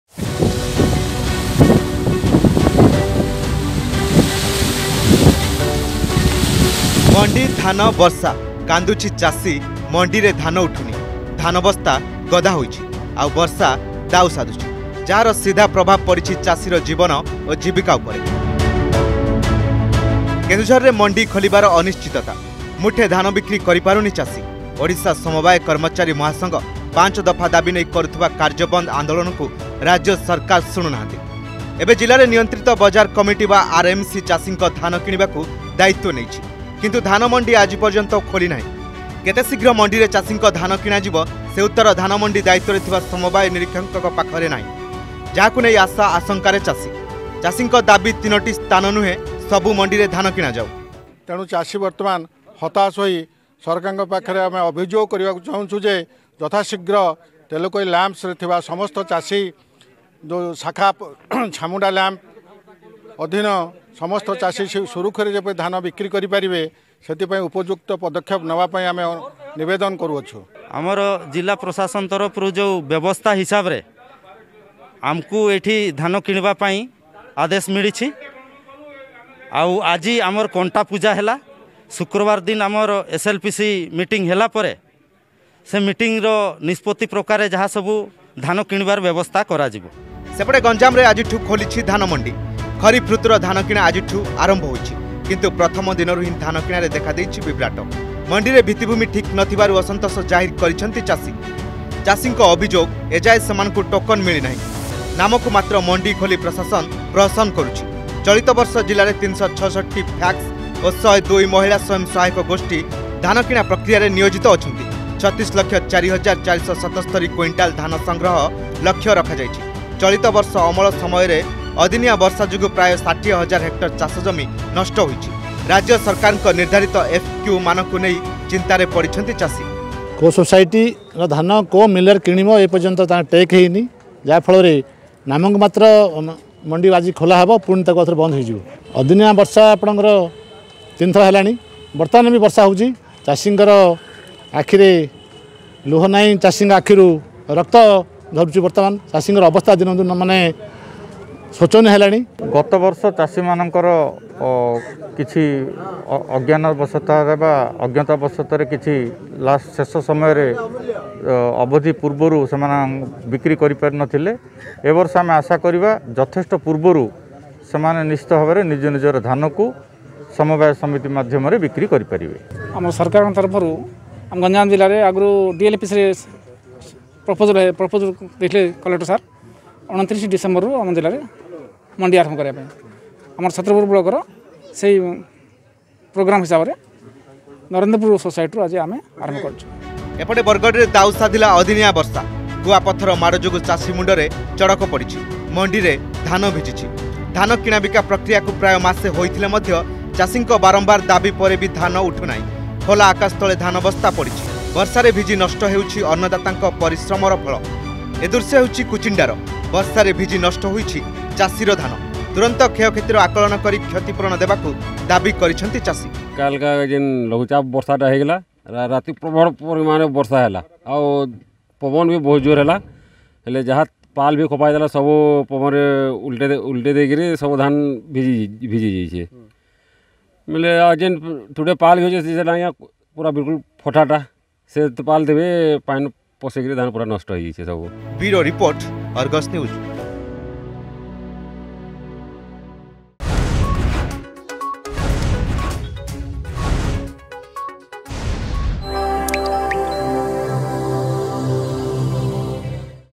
ମଣ଼୍ଡି, ଧାନ, ବର୍ଷା : କାନ୍ଦୁଛି ଚାଷୀ
ଚାଷୀର ଜୀବନ ଜୀବିକାକୁ ନେଇ ବଢିଲା ଜଞ୍ଜାଳ। ମଣ୍ଡିରେ ଧାନ ନ ଉଠିବାରୁ କାନ୍ଦୁଛି ଚାଷୀ। ସେପଟେ ଦାଉ ସାଧିଛି ବର୍ଷା।  ବିଭିନ୍ନ ସ୍ଥାନରେ ଧାନ କିଣାରେ ବିଭ୍ରାଟ  ଯୋଗୁଁ ଚିନ୍ତାରେ ଚାଷୀ।